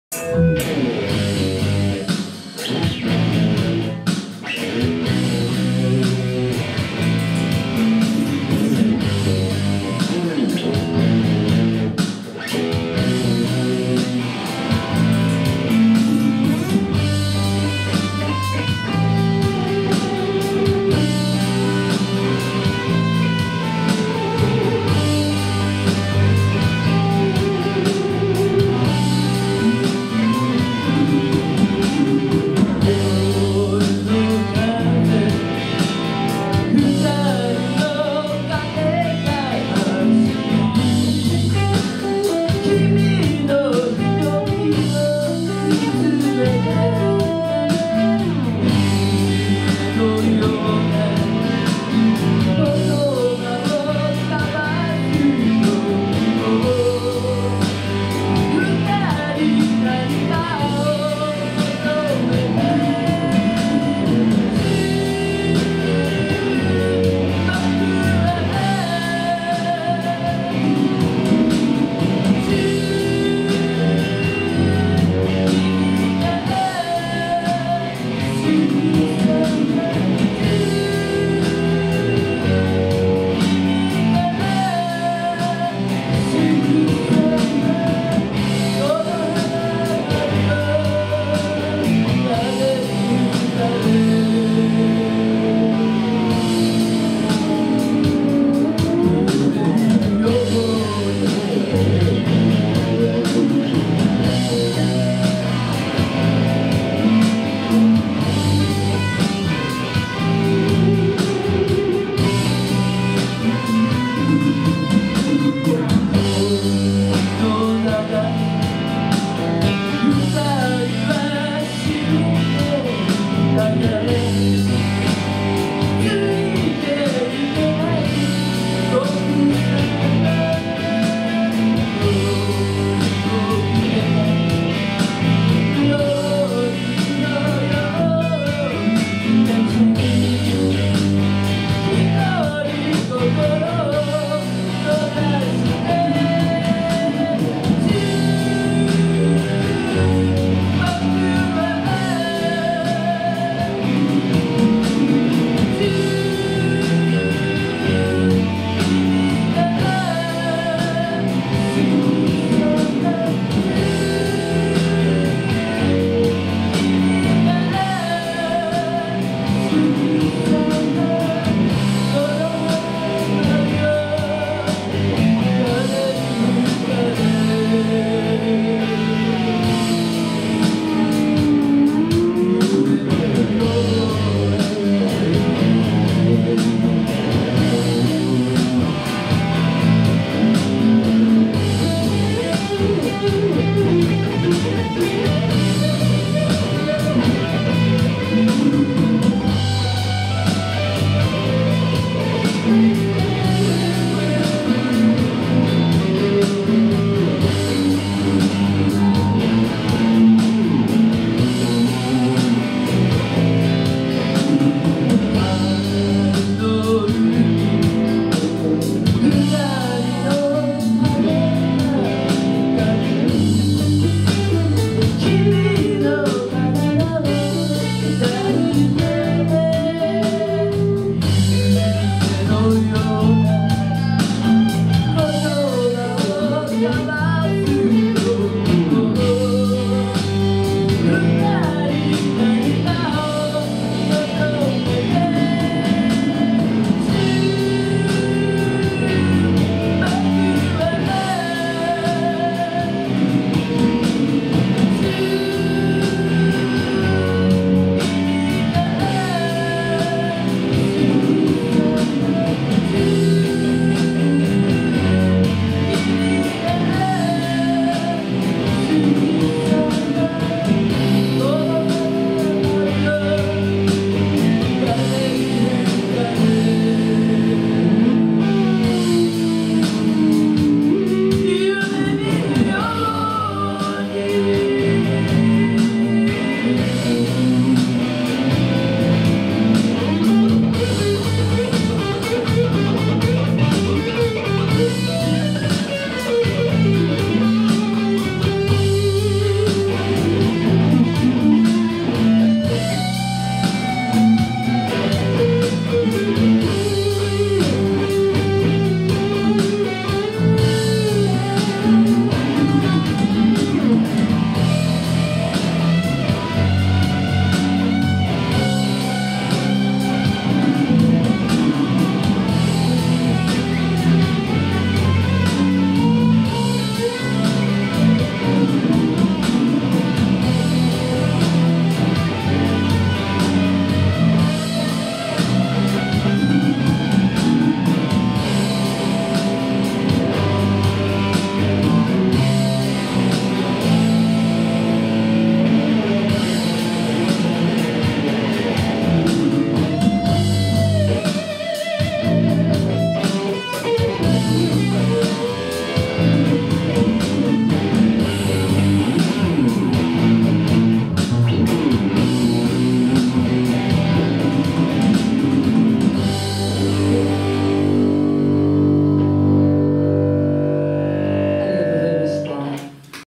JUN 2
2カメを織り交ぜて再編集のベストテイクですね。
ムスタングが欲しいと思ってくれる人がいたら大成功。
67年ムスタングいいですよ、今がヴィンテージを入手するチャンスかもね。今後は、さらに値上がり間違いなしですね。4分50秒からの音は、完璧ですね。